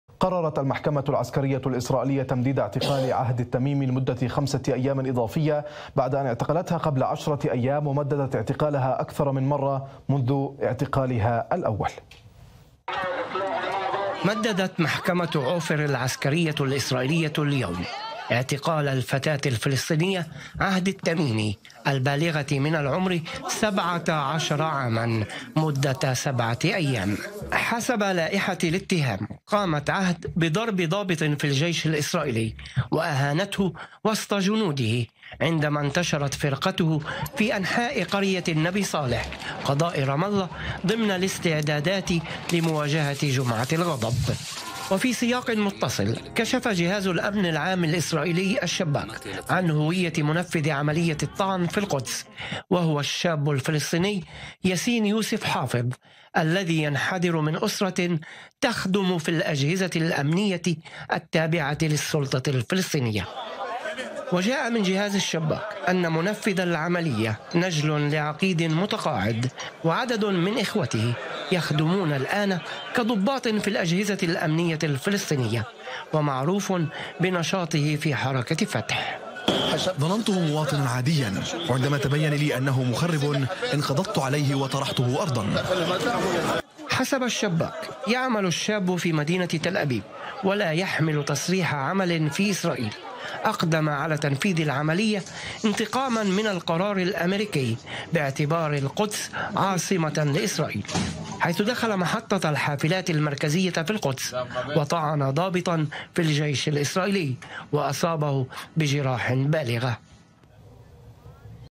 تقرير: المحكمة العسكرية الاسرائيلية تمدد اعتقال عهد التميمي
أخبار الساعة | تقديم: مهند نبيل | تقرير: مرعي مرعي